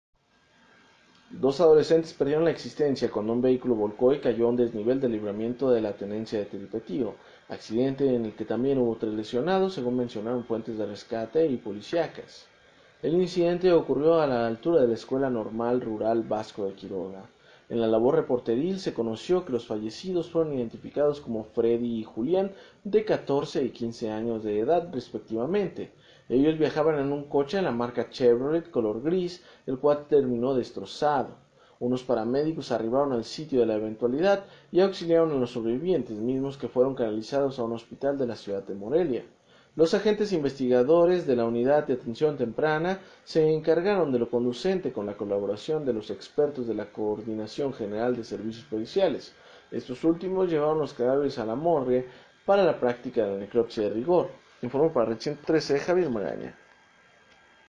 Mueren 2 adolescentes tras accidente automovilístico en Tiripetío
Morelia, Mich.- Domingo 30 de junio de 2019.- Dos adolescentes perdieron la existencia cuando un vehículo volcó y cayó a un desnivel del Libramiento de la Tenencia Tiripetío, accidente en el que también hubo tres lesionados, mencionaron fuentes de rescate y policiales.

El percance se registró la tarde-noche de este domingo, a la altura de la población de las instalaciones de la Escuela Normal Rural Vasco de Quiroga.

En la labor reporteril se conoció que los fallecidos fueron identificados como Freddy y Julián, de 14 y 15 años de edad, respectivamente, ellos viajaban en un coche de la marca Chevrolet, color gris, el cual terminó destrozado.

Unos paramédicos arribaron al sitio de la eventualidad y auxiliaron a los sobrevivientes, mismos que fueron canalizados a un hospital de la ciudad de Morelia.

Los agentes investigadores de la Unidad de Atención Temprana se encargaron de lo conducente con la colaboración de los expertos de la Coordinación General de Servicios Periciales, éstos últimos llevaron los cadáveres a la morgue para la práctica de la necropsia de rigor.